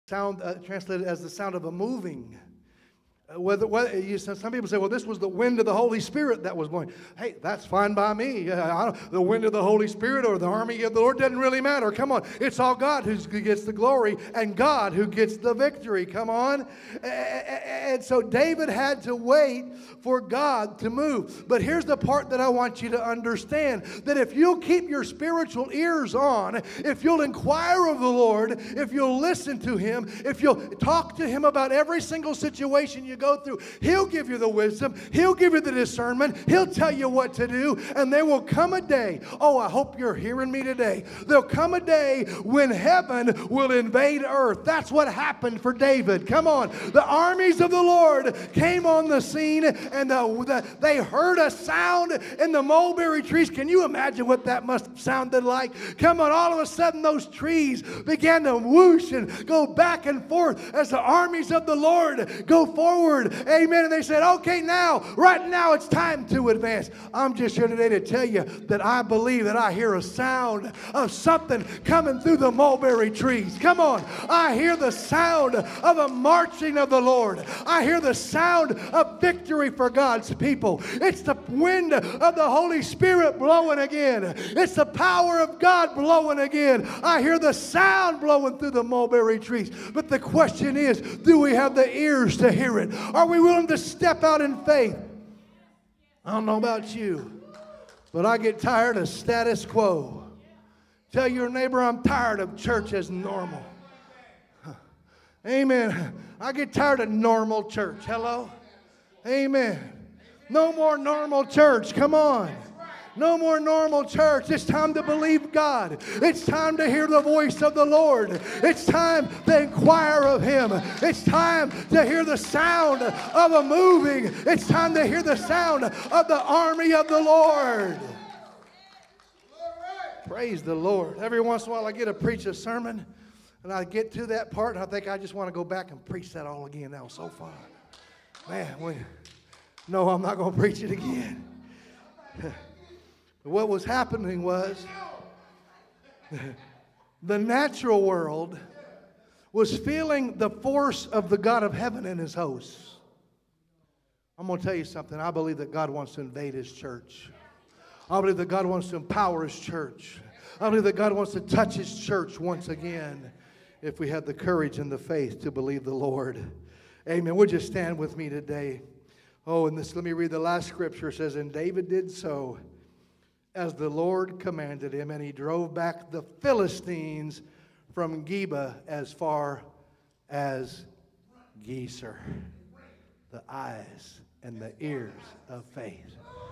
The Eyes and Ears of Faith Conclusion
Are you ready to advance when you hear the sound of a moving in your life? Like David of old we must have our ears of faith on listening what the Spirit says to the church.